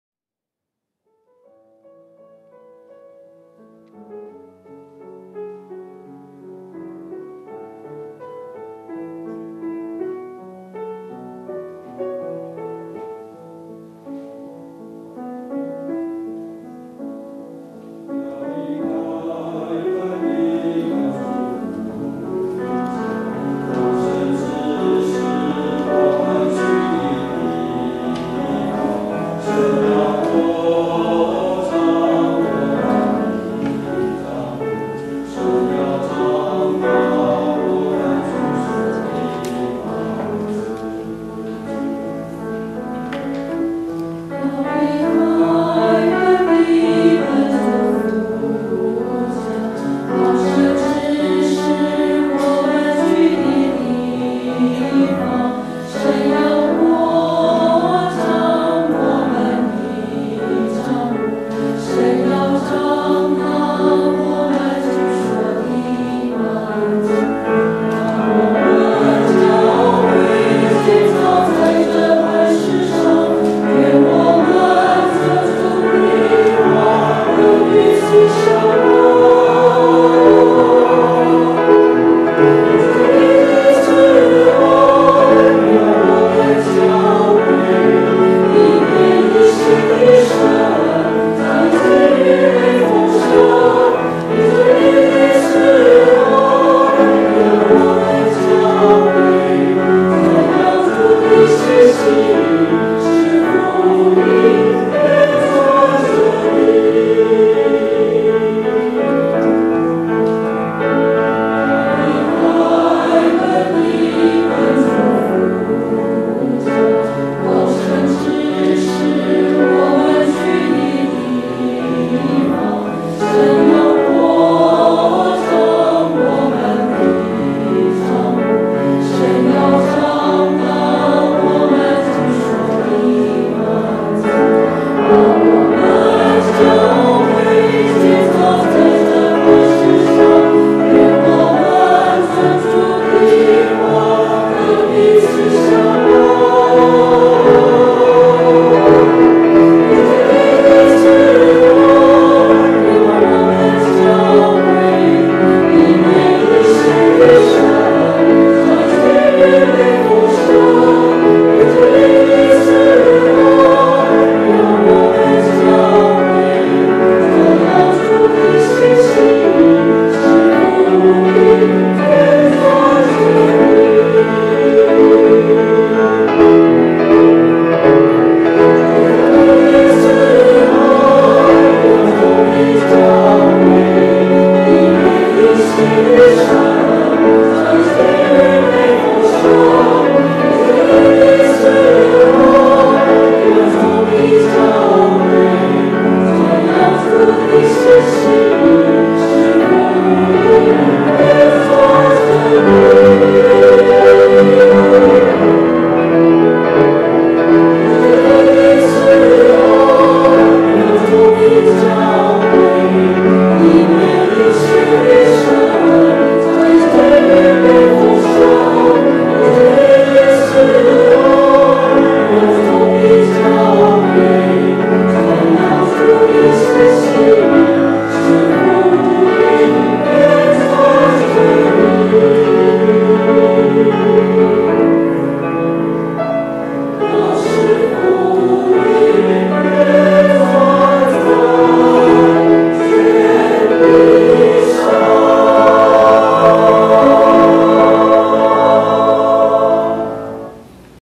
中壢教會聖歌隊獻詩
2015年3月22日台灣基督長老教會中壢教會台語禮拜獻詩錄影